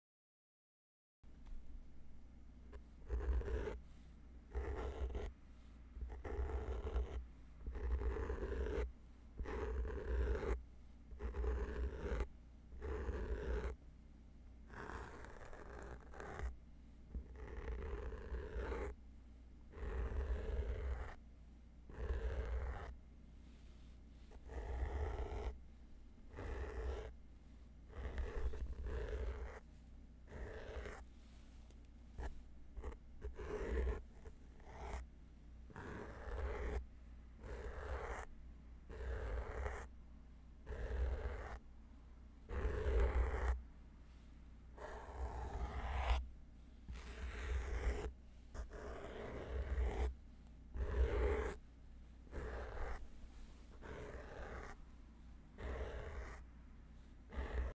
【ASMR】脳に響くゴリゴリ音 × お洒落なキャンドル空間
特殊係ASMRです！😝
お洒落な空間の中で脳に響くゴリゴリ音をお楽しみください！！

「オシャレなASMRを皆様に」届けるため日々精進していきますので